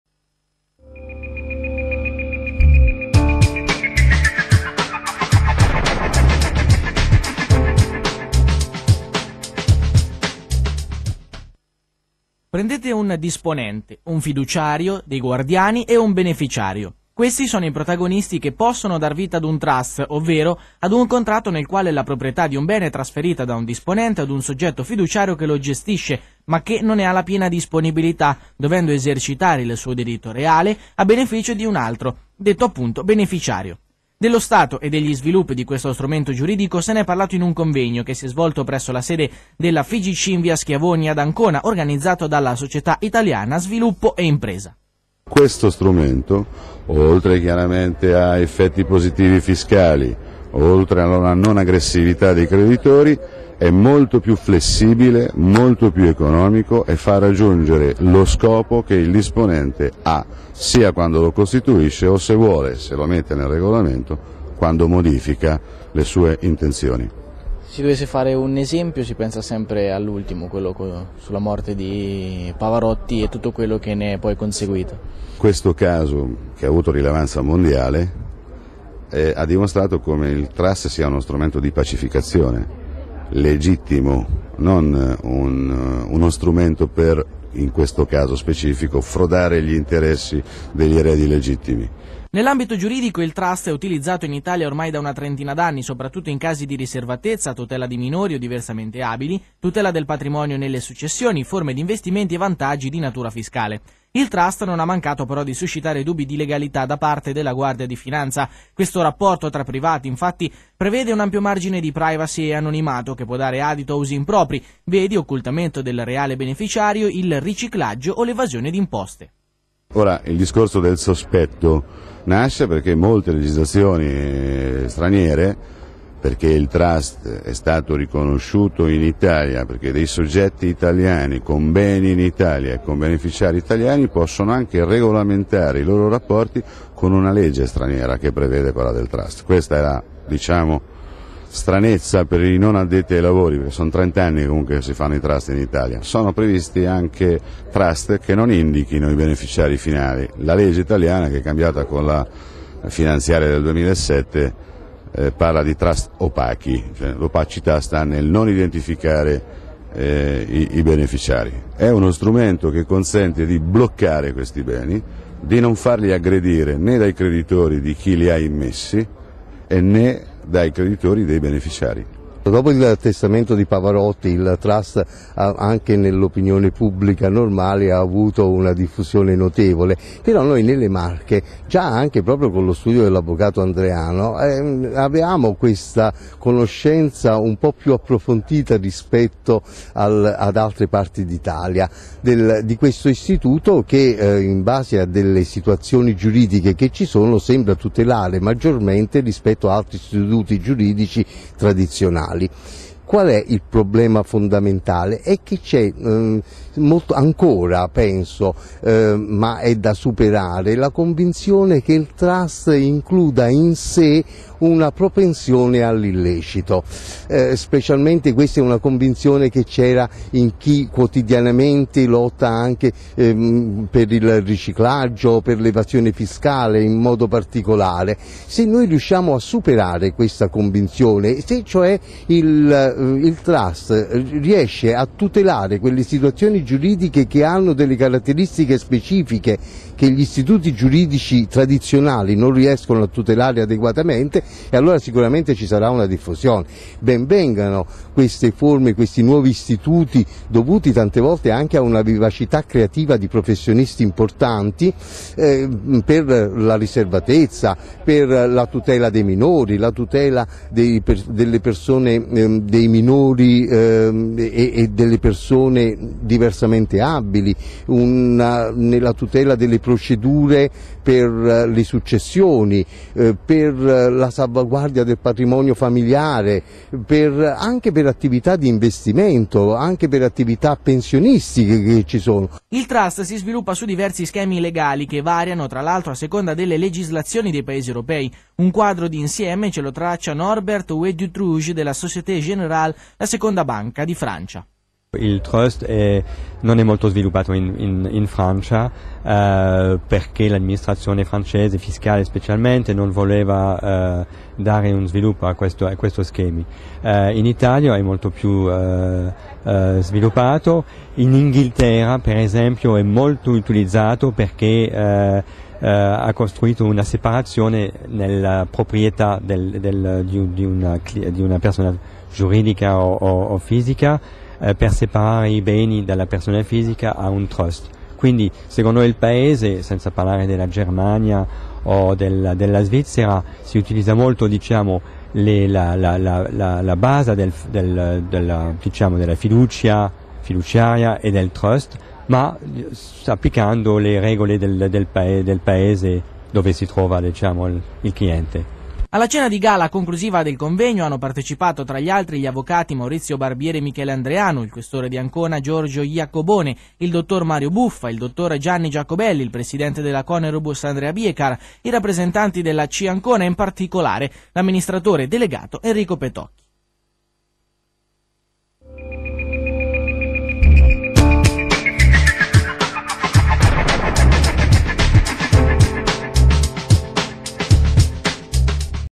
SIS&I Convegno Trust 2008 servizio ètv
Servizio di ètv sul convegno organizzato dalla SIS&I: Stato e Sviluppi dei Trust in Italia del 19/20 Settembre 2008